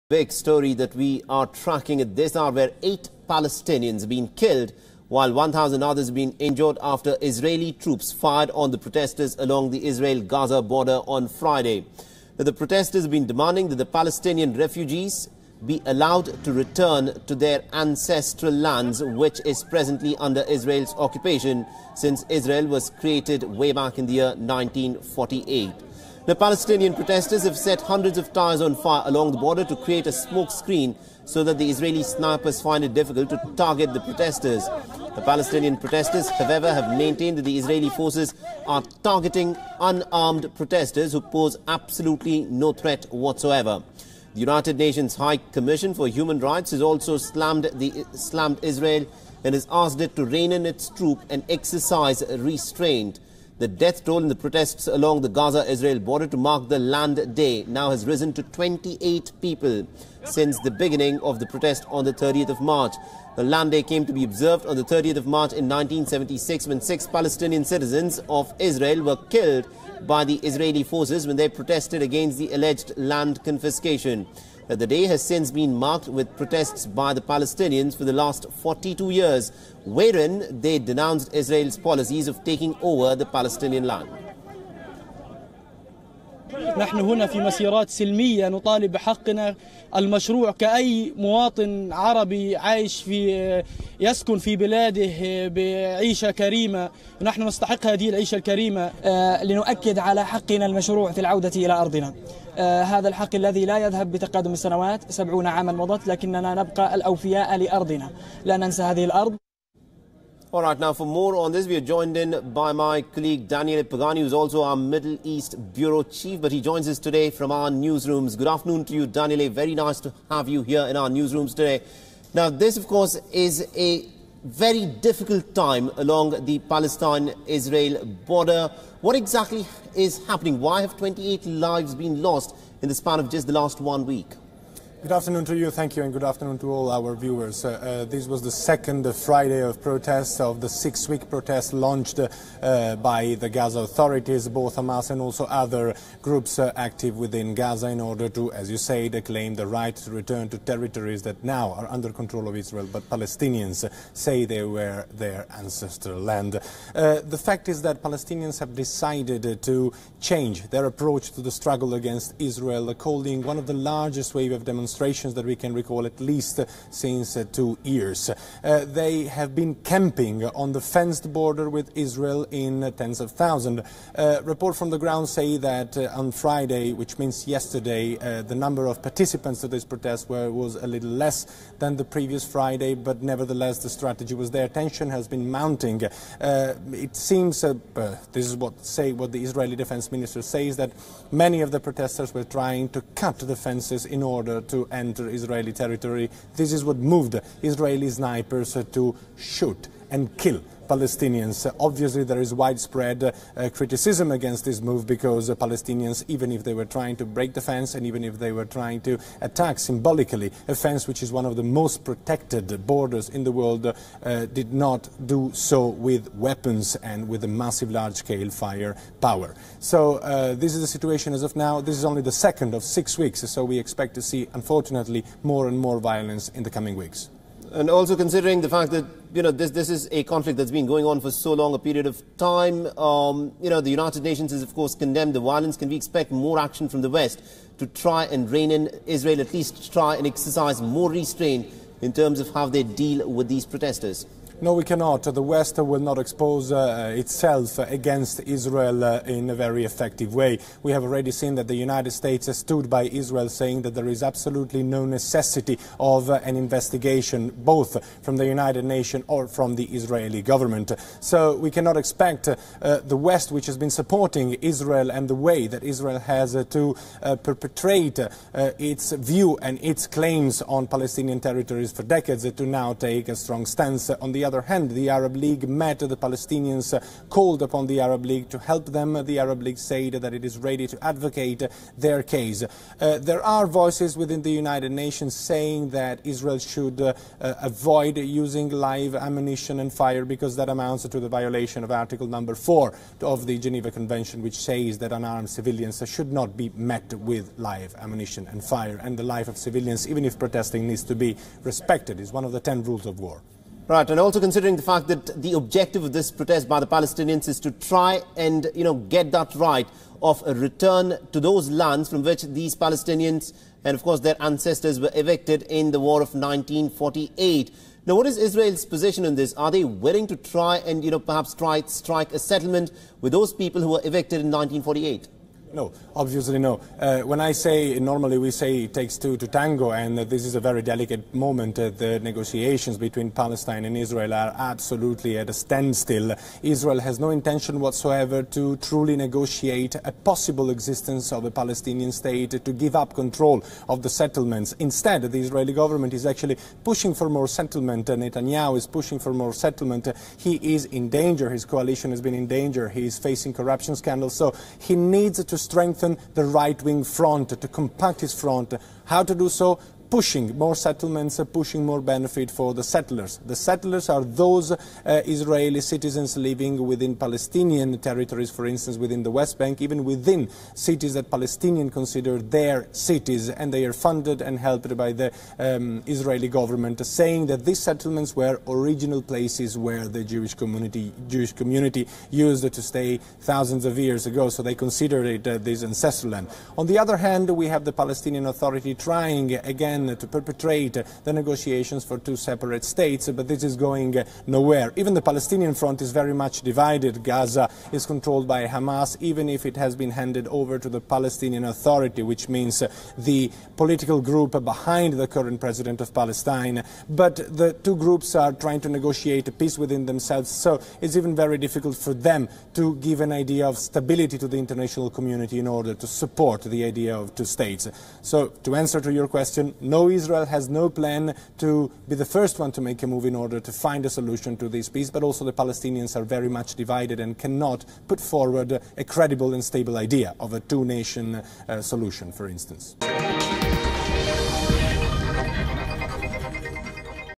Protests along Ghaza- Israel border resumes: Protestors demand return to their homeland
8 Palestinians have been killed and 1000's have been wounded in the Ghaza- Israel border protests. The protestors demand return to their homeland.